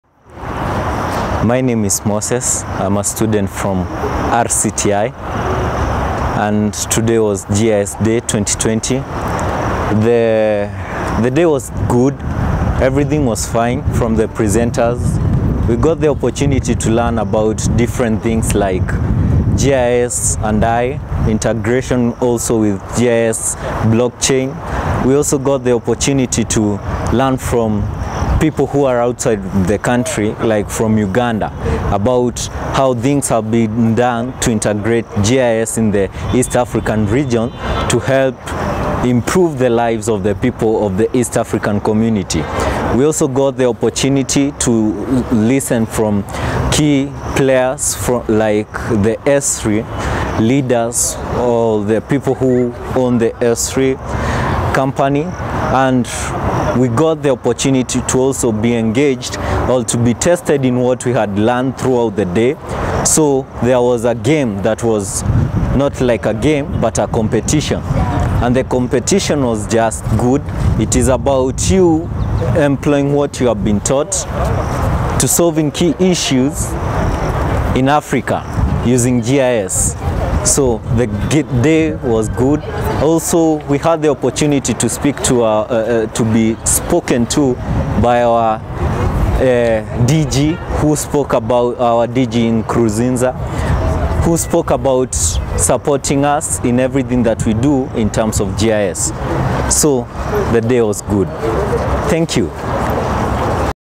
GIS DAY EXPERIENCE
On 18th November 2020, Geographers celebrated GIS Day with an aim of inspiring the use of GIS and  increasing geo awareness. After a day full of inspiring activities, here is what one of the participants had to say.